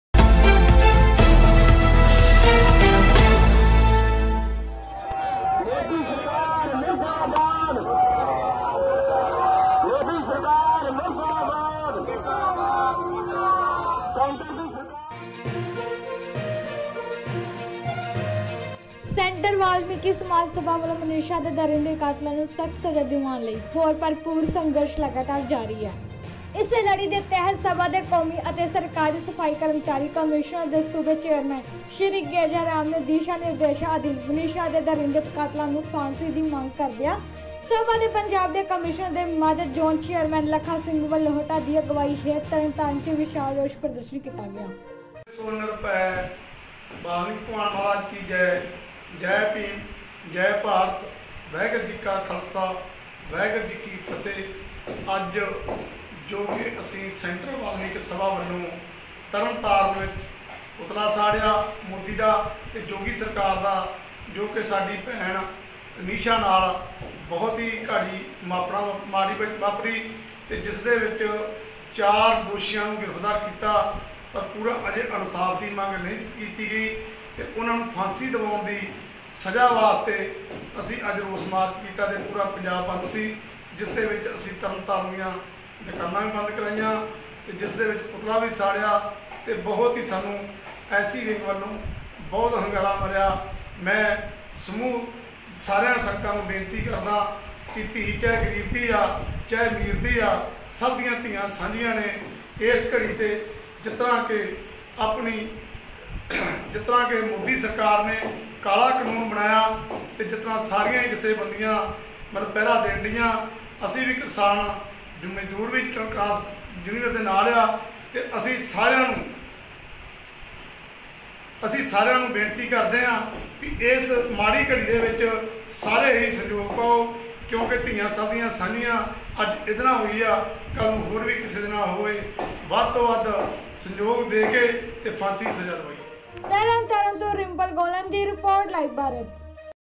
ਮਨੀਸ਼ਾ ਦੇ ਦਰਿੰਦੇ ਕਾਤਲਾਂ ਨੂੰ ਲਗਾਓ ਫਾਂਸੀ ਵਾਲਮੀਕਿ ਸਭਾ ਨੇ ਫੂਕਿਆ ਮੋਦੀ-ਯੋਗੀ ਸਰਕਾਰ ਦਾ ਪੁਤਲਾ
latest news
punjab news